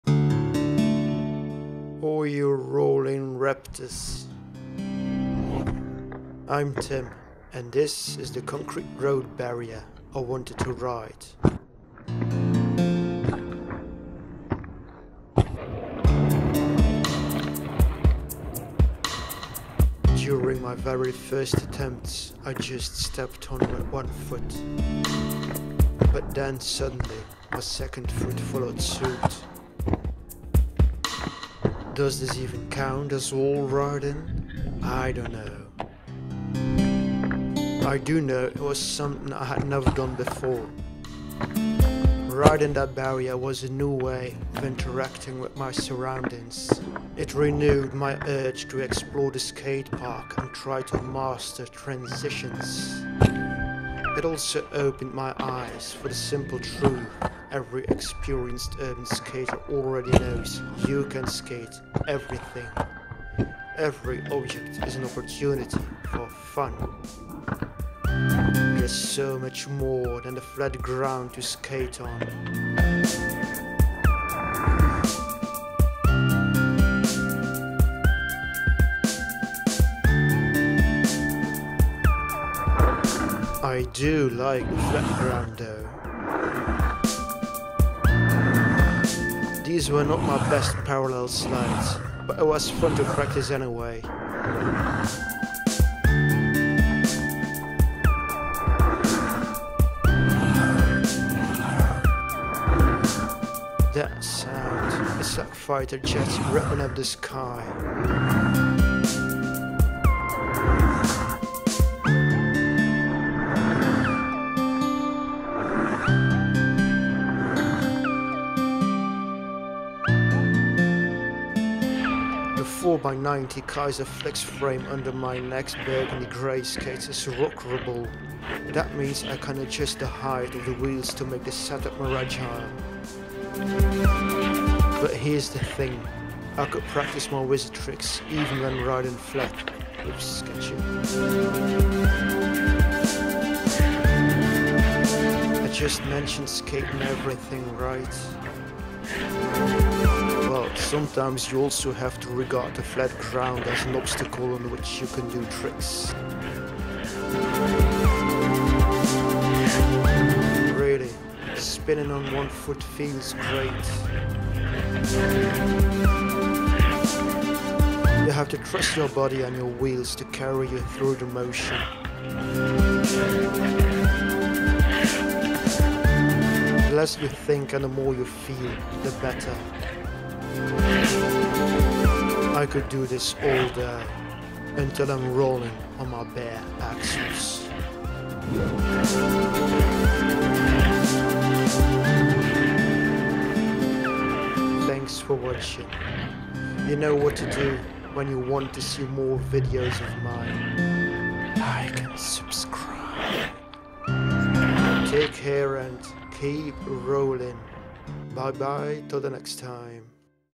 Getting better (a lot) on inline skates | Powerslide Next 90 | Rollerblading
A concrete road barrier is a great place to start wall riding. And to my surprise, it worked. Then I did some parallel slides (not my best) and some flatland tricks.

This may be my best video to date. I tried some B-roll stuff I’ve never tried before. The Osmo Action is a great camera.

0:00 Wall Riding on a Concrete Road Barrier
1:21 Parallel Slides
2:01 Wizard Skating on a Flat Setup

🔗 LINKS

🛼 MY SETUP

Frame: Kizer Flux 2in1 (nylon PA 66 plus glass-fiber)
Powerslide Spinner wheels 90 mm 88A
Wicked ABEC 9 bearings

🎥 CAMERA USED

✂️ EDITED IN

🎵 MUSIC

23/5/2021, Gent